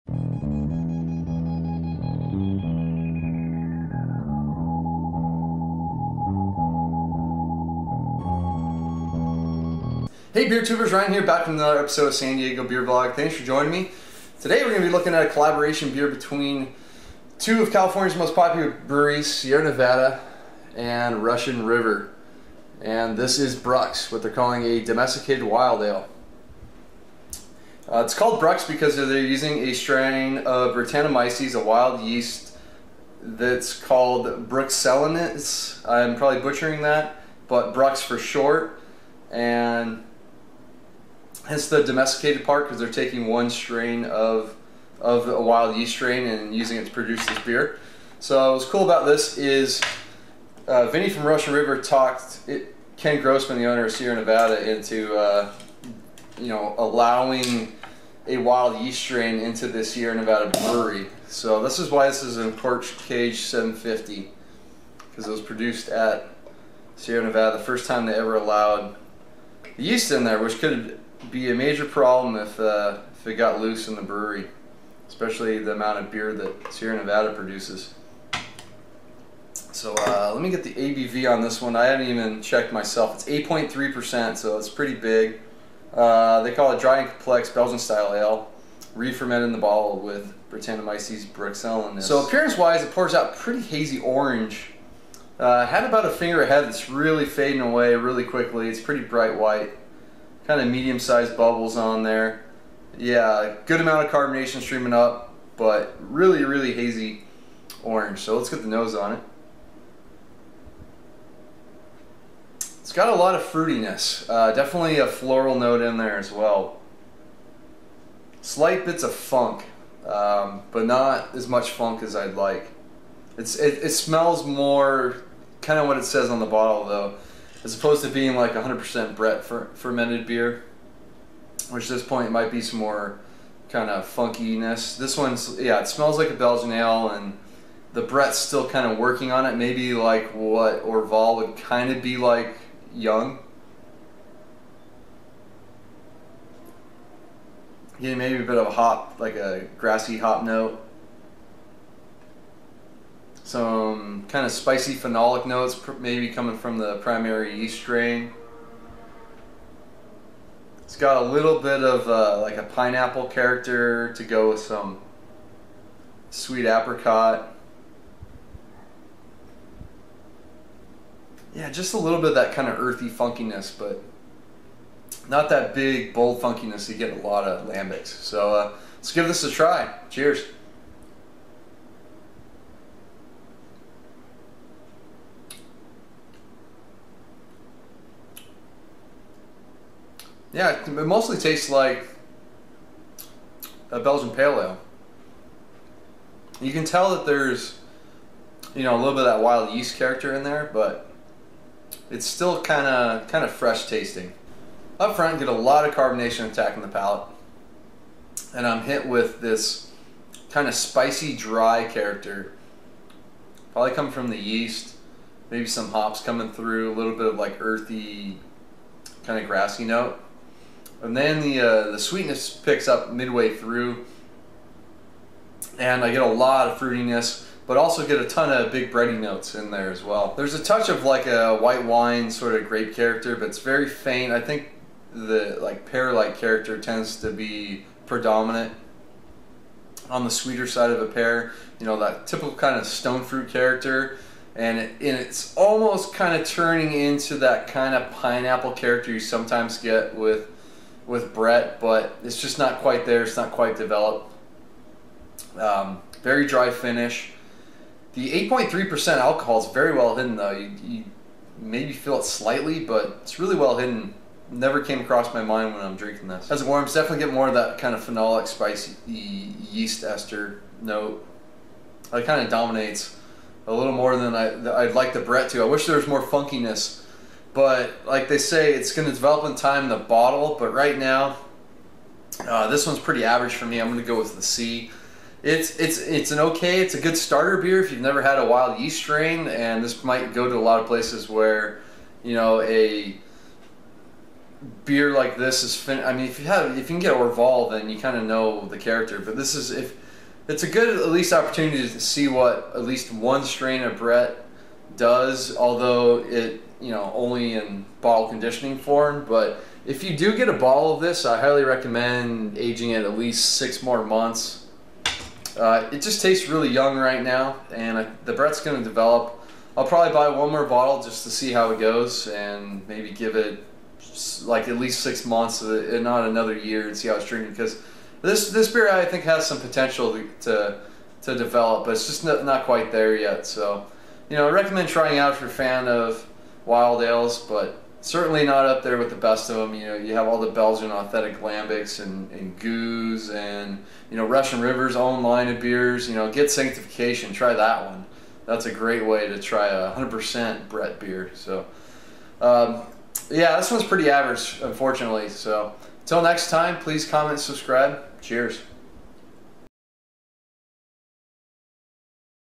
Sierra Nevada & Russian River BRUX Wild Ale Video Beer Review | San Diego Beer Vlog EP 330
Sierra Nevada & Russian River BRUX Domesticated Wild Ale, 8.3% ABV

"What began as mutual admiration between Sierra Nevada's Grossman Family and Russian River's Cilurzos has grown and progressed into genuine friendship. Brux began as an idea and has grown into something altogether different:

The liquid manifestation of change over time.

Refermented in the bottle with Brettanomyces bruxellensis, Brux will change and develop over time. Copper-colored, dry and complex, with slightly tart notes of green grass, pear, spice and lemon - this ale will progress in the bottle for many years."